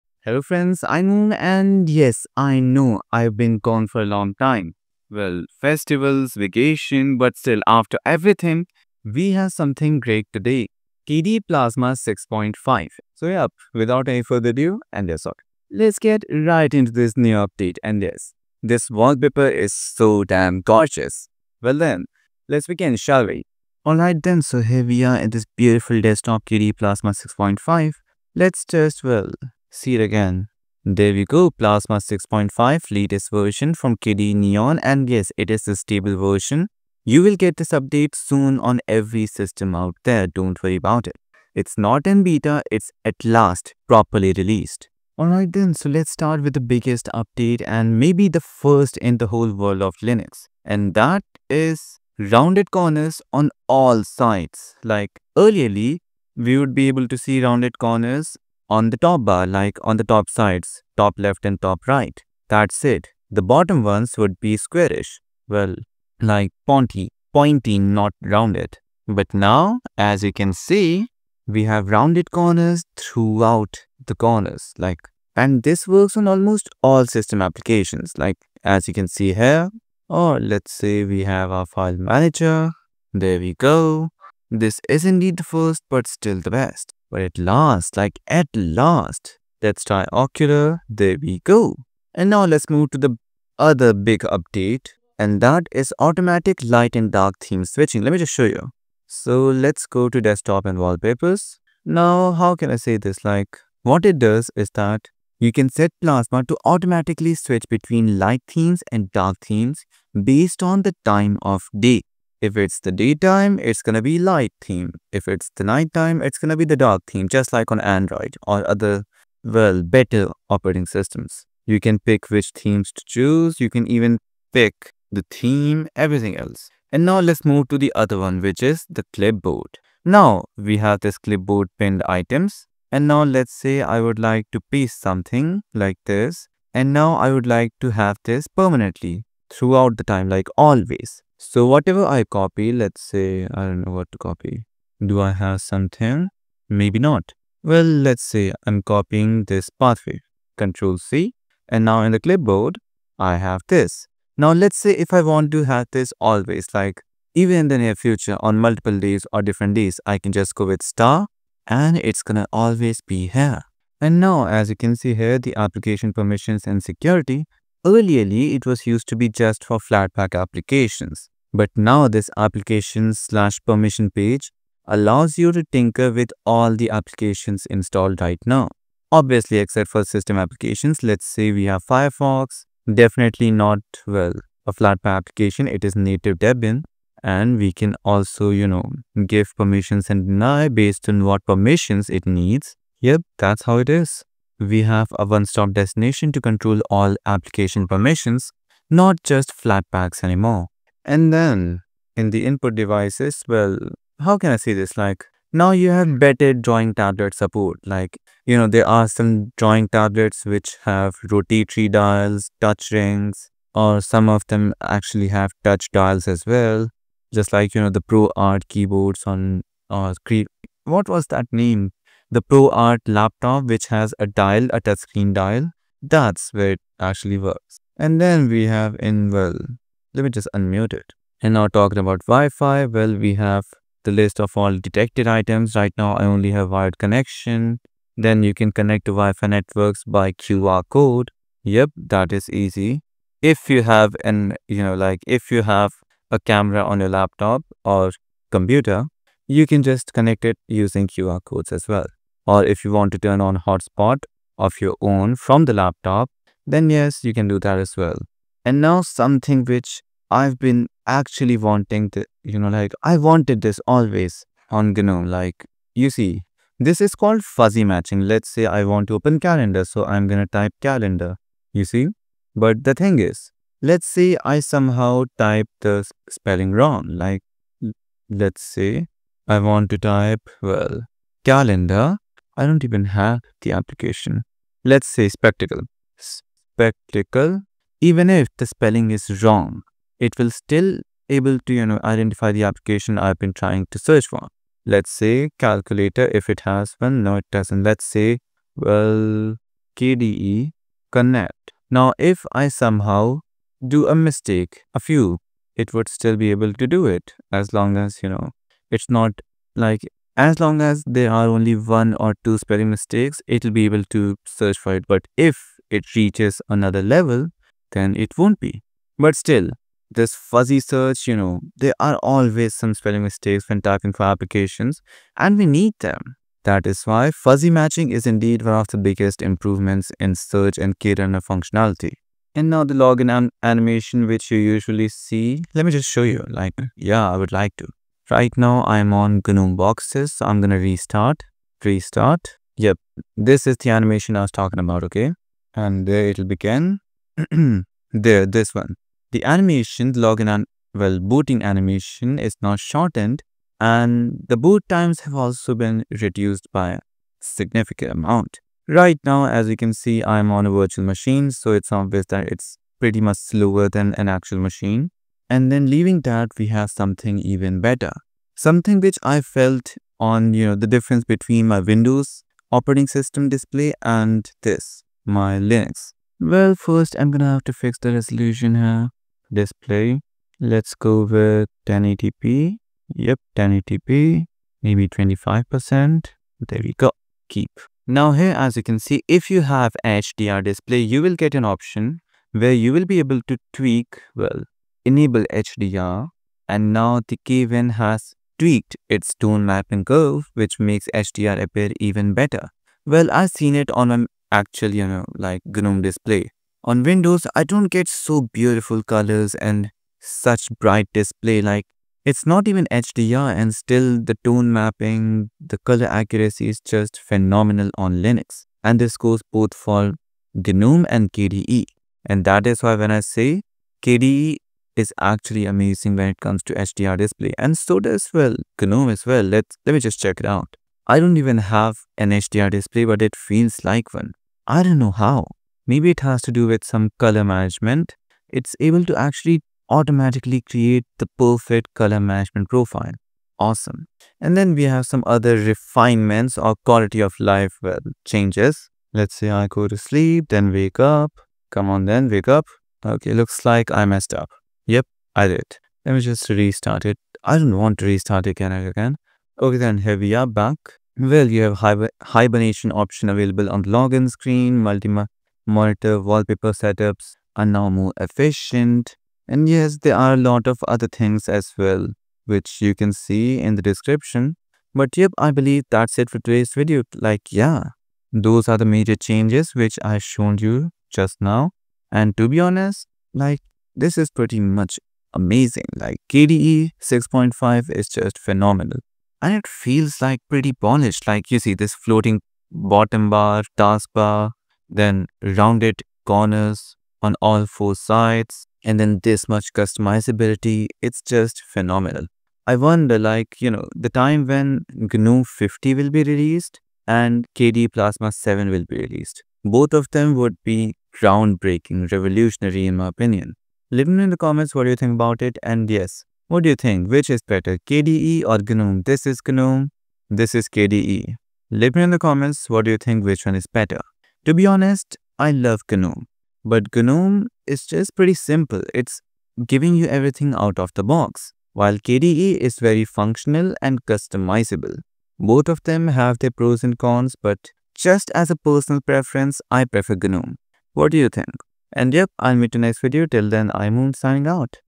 KDE Plasma 6.5 - New Updates & Features
KDE Plasma 6.5 is finally here — and it’s one of the most refined updates in the Plasma 6 series.
Instead of huge redesigns, this version focuses on polish, smoothness, and smarter details across the desktop.

In this video, we explore all the major changes, new features, and hidden tweaks that make Plasma 6.5 a delight to use.

🔹 1. Visual Improvements

Windows now have rounded bottom corners, giving a modern and consistent look.
The automatic light/dark theme switch lets your system change appearance with time of day — including wallpapers!

🔹 2. Smarter Clipboard & Remote Desktop

You can now pin clipboard items to keep them permanently accessible.
The RDP server supports clipboard sharing and uses your existing user account — no more extra logins.
Mute shortcuts now affect all microphones, and volume adjustments unmute all playback devices automatically.

🔹 3. Settings & Widgets

A new Application Permissions page replaces the old Flatpak settings — now works for all apps.
A dedicated Drawing Tablet section supports touch rings and dials.
Redesigned Network page allows connecting directly from settings.
KRunner search adds fuzzy matching and instant results.
Sticky Notes widget now supports smaller sizes, colours, and transparency.

🔹 4. Wayland & Performance Upgrades

Overlay planes reduce CPU usage and improve battery life when playing videos.
HDR rendering looks better with improved tone-mapping.
Wayland gains experimental Picture-in-Picture support and faster startup animations.

🔹 5. Quality of Life Tweaks

Hibernate option on login screen.
Lower memory use for multi-monitor wallpapers.
Keyboard shortcuts can advance wallpaper slideshows.
Digital clock calendar redesign and new printer toner alerts.

🔹 6. Accessibility Enhancements

Caps Lock state is announced by screen readers.
New grayscale filter and zoom improvements.
Audio-visual alerts now assist users with hearing or visual sensitivity.

🔹 7. Discover & App Updates

Discover launches faster and supports flatpak+https:// URLs for easy Flathub installs.
Emoji picker now shows all emojis by default and improves search behaviour.
Spectacle now captures pop-ups during screen recordings.

🕓 Availability

KDE Plasma 6.5 will arrive soon in your distro’s repositories.
If you’re impatient, you can try it right now using KDE’s live images or testing builds.

In short: KDE Plasma 6.5 is all about refinement — smoother visuals, smarter features, better performance, and a cleaner experience overall.
If you enjoy a sleek and powerful Linux desktop, this update is a must-try.